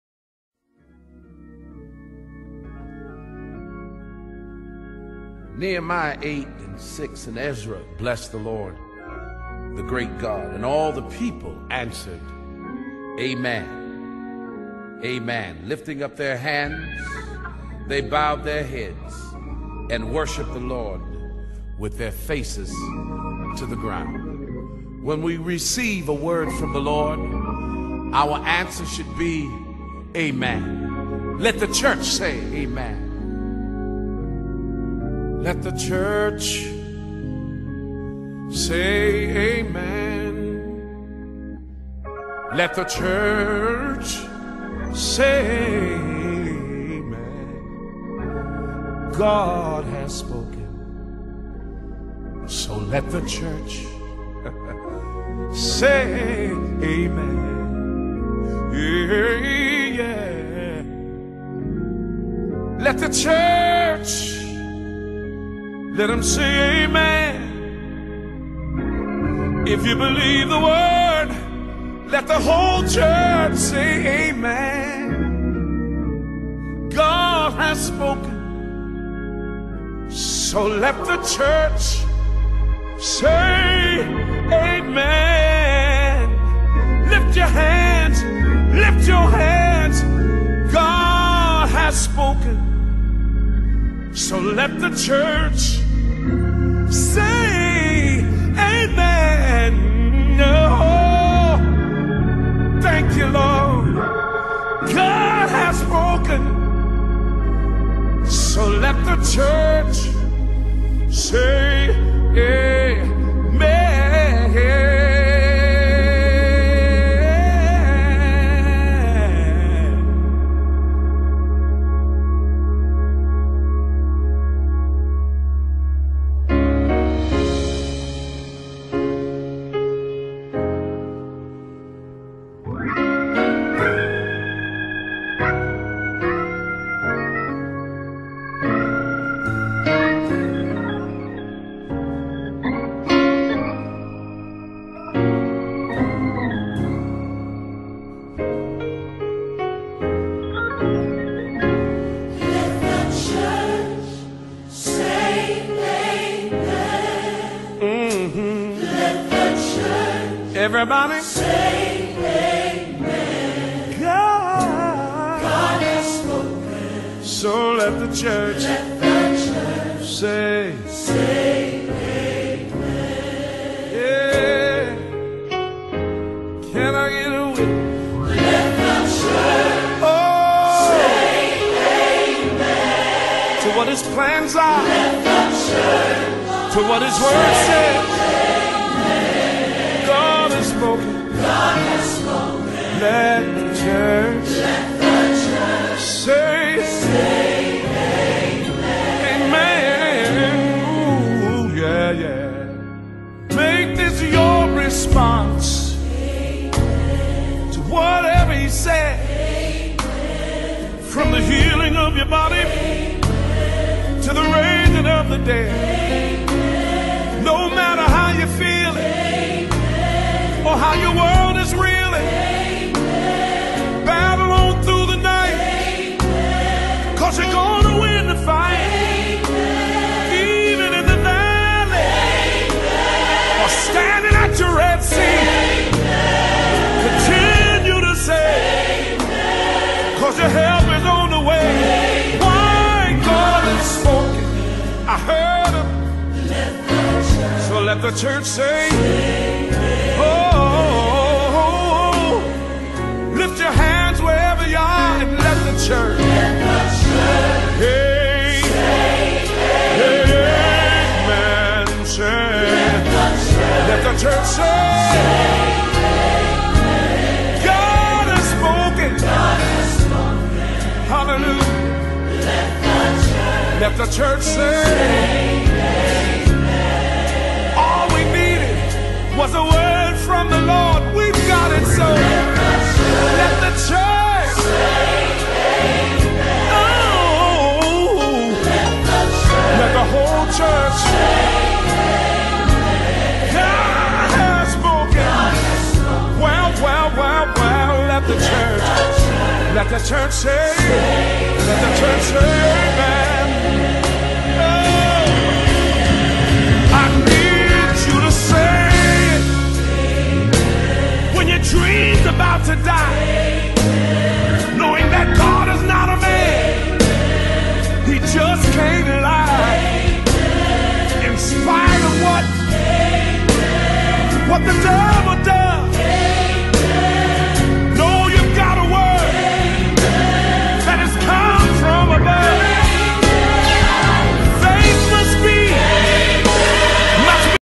New Generation Ministries at the Metroplex - Sight and Sound Bus Trip
Bishop Charles S. Waters and the congregation paid a visit to Sight and Sound in Lancaster County, PA to experience Jonah!!